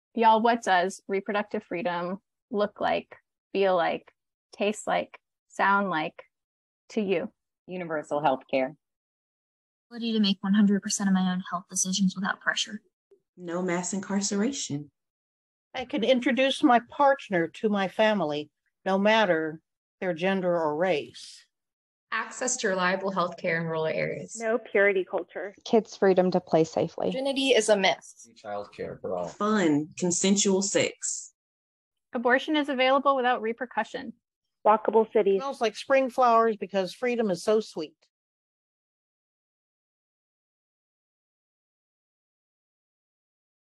Our Visions for Reproductive Freedom
Your favorite abortion advocates are formally introducing the Reproductive Freedom Act in Georgia in 2023!

To hop on board, sign the petition You can also make your own short expressing your vision for Reproductive Freedom